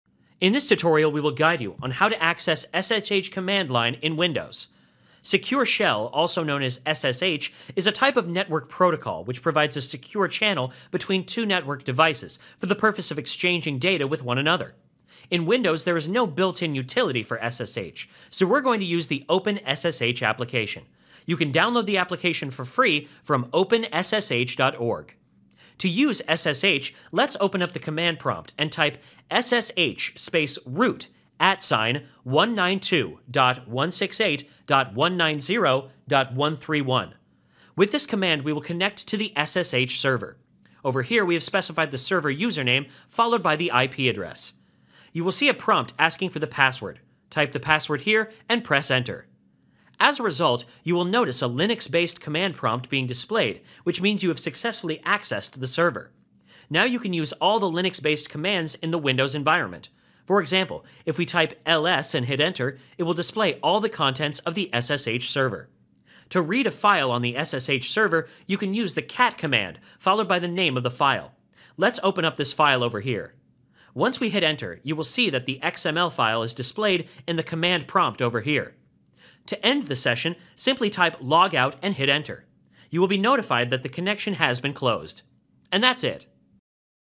How to Access SSH Command Line in Windows
In this tutorial we will guide you on how to access windows SSH command line.

Follow this easy step by step tutorial to learn about SSH, and how to use SSH command line in windows to access SSH server. SSH, or more accurately, Secure Shell is network protocol which provides a set of rules for a secure communications between two computers connected by a medium prone to outside threats. In windows there is no built-in utility for SSH, so we are going to use the open ssh application. You can download the application for free from openssh.org.

Step #1: Getting started with SSH

To use SSH, let's open up the command prompt and type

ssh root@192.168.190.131

With this command we will connect to the SSH server. Over here, we have specified the server username followed by the IP address.

Step #2: Authentication

You will see a prompt asking for the password. Type the password here and press Enter.

Step #3: SSH Server

As a result, you will notice a Linux based command prompt being displayed which means you have successfully accessed the server using windows command line ssh.

Step #4: Accessing SSH Server

Now, you can use all the Linux based commands in the windows environment. For example, if we type ls and hit enter, it will display all the contents of the SSH server.

Step #5: Using SSH Server Resources

To read a file on the SSH server, you can use the cat command followed by the name of the file. Let's open up this file over here. Once we hit enter, you will see that the xml file is displayed in the command prompt over here.

Step #7: Terminating Session

To end the session of the windows command line ssh, simply type logout and hit enter. You will be notified that the connection has been closed.

And that is how windows SSH command line is used to access the SSH server, execute commands and use its resources in a secure manner.